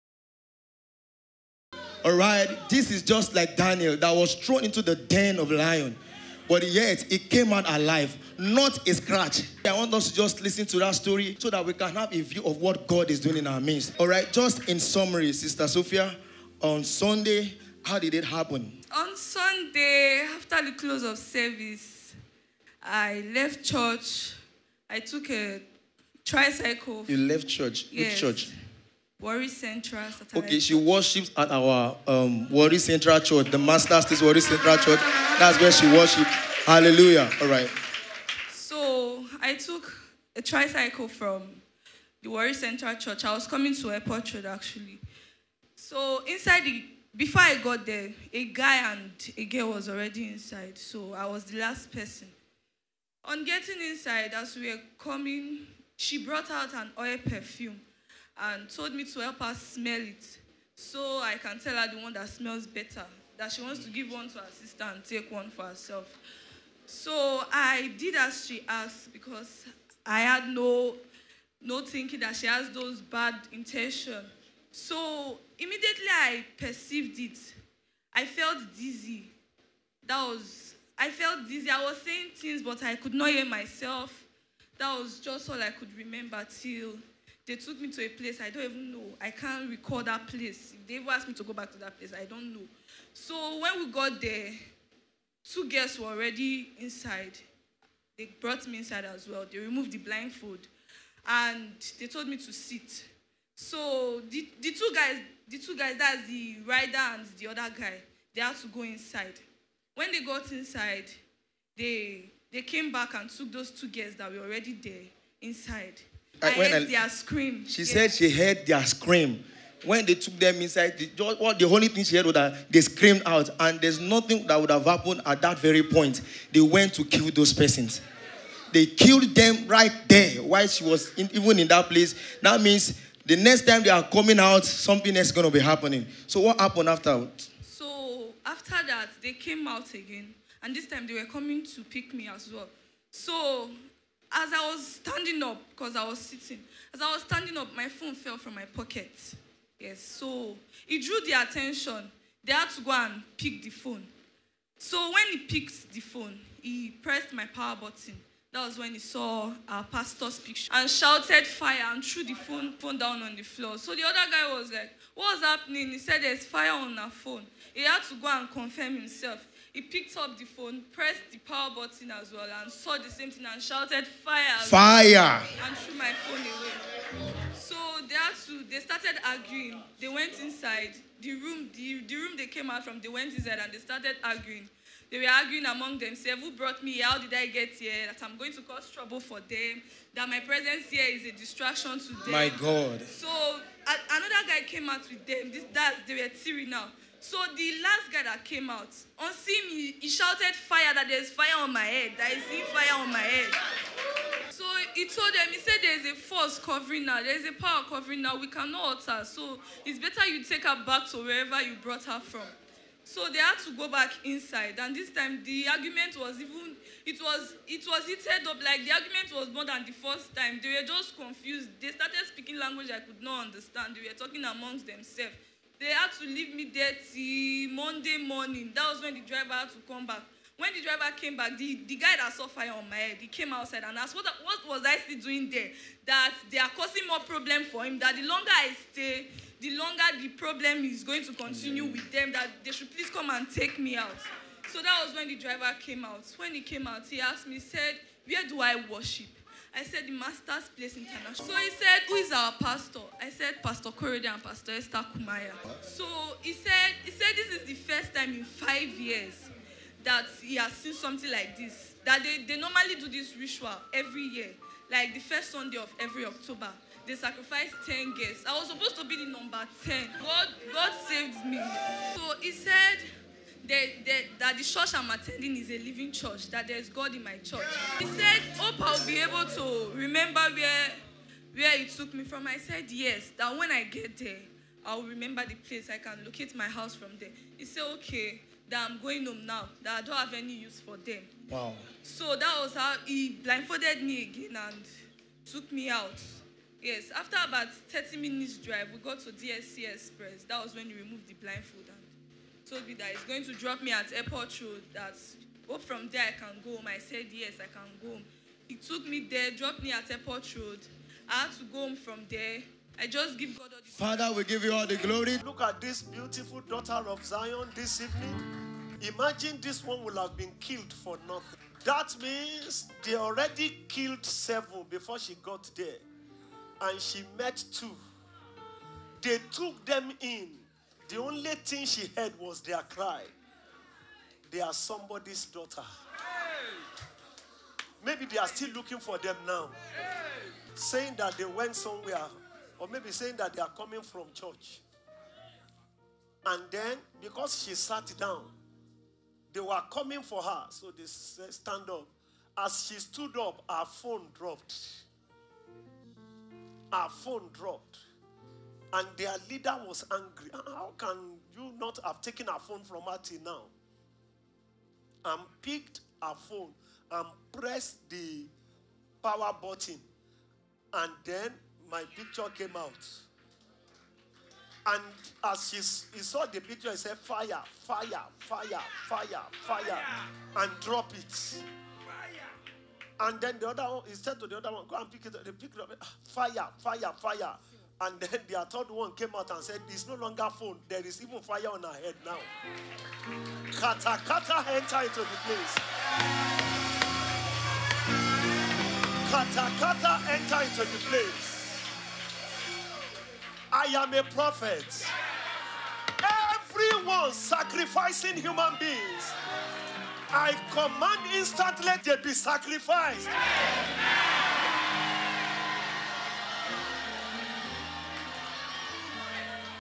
RESCUED FROM RITUAL KILLERS AFTER 9 GIRLS GOT KILLED
Hear how this young lady escaped from the den of Ritualist after 9 other girls had been killed! JESUS is still working amazing miracles in our time and days!